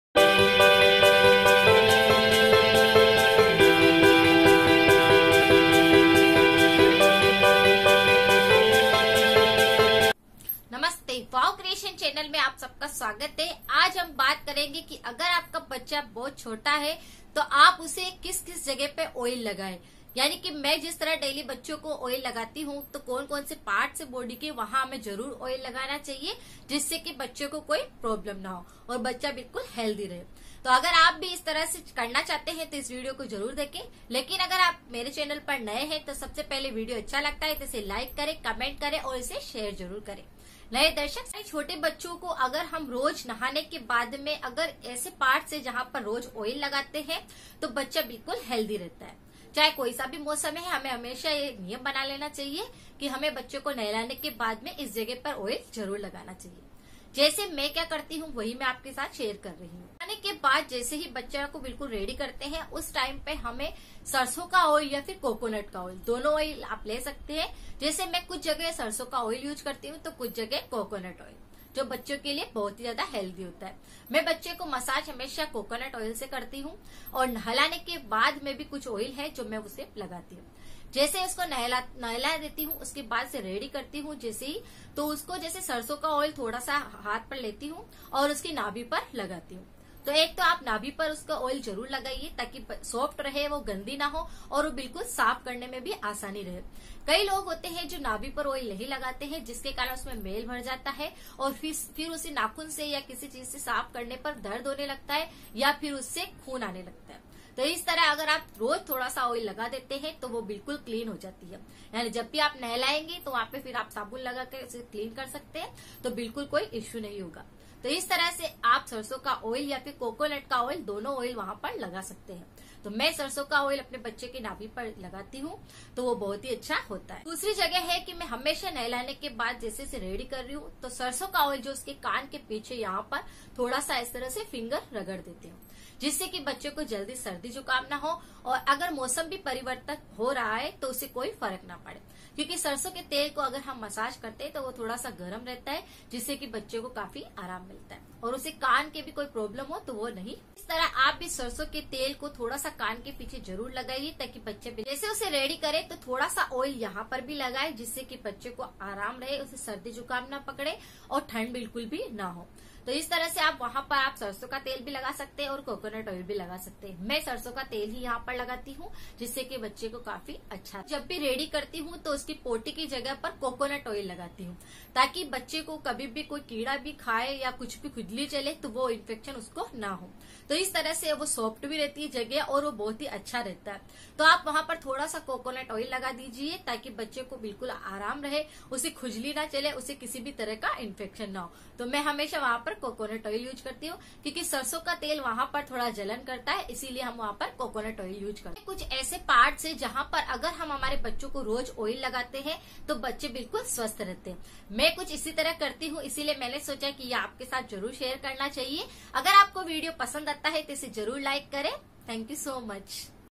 BABY CARE baccho ko ready krte time kre ye kaam taki bacche rhe savsth
छोटे बच्चे बहुत ही मासूम होते है,उनकी मासूमियत से बच्चे हमारा दिल जीत लेते है उनको कभी कोई भी समस्या न हो इसलिए माता पिता उनके लिए हमेशा फिक्रमंद रहते है लेकिन जब भी माताए अपने बच्चो को तैयार करे यानी कि उनको जब भी नहला कर तैयार करे तो कुछ चीजें अगर माँ करे तो बच्चो को फिर कोई समस्या नंगी होती वो हमेशा खिल खिलाते रहते है जैसे कोकोनट तैल को पोटी की जगह,सरसो तैल को कान के पीछे, नाक में कुछ बूंद,ओर सर पर लगाये तो सर्दी से बचाव होता है नाभि में रोज तैल लगाने से बच्चो की नाभि हमेशा साफ रहती है इस वीडियो में बताया है सभी चीजें जिसे आप कर के वच्चो को ठीक रख सकते है।